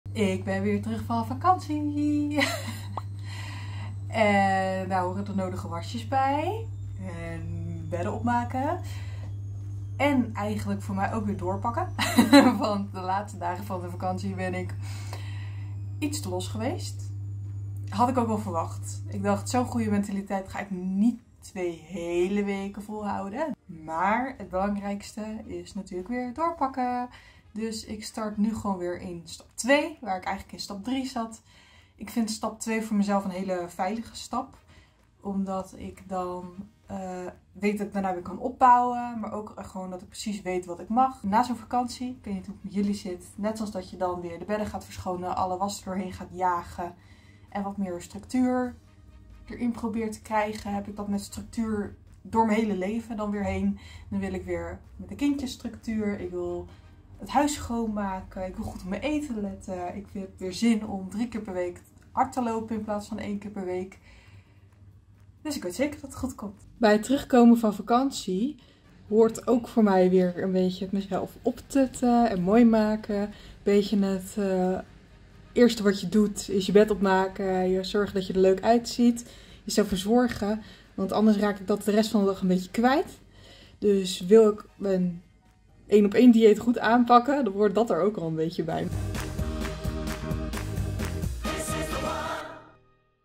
Vlog 6 - Zoë is terug van vakantie en heeft behoefte aan structuur: ‘Ik heb er weer zin in’
Zoë is terug van vakantie en heeft weer behoefte aan structuur in haar leven. Doorpakken dus! Hoe zo dat doet? Check snel haar nieuwe vlog.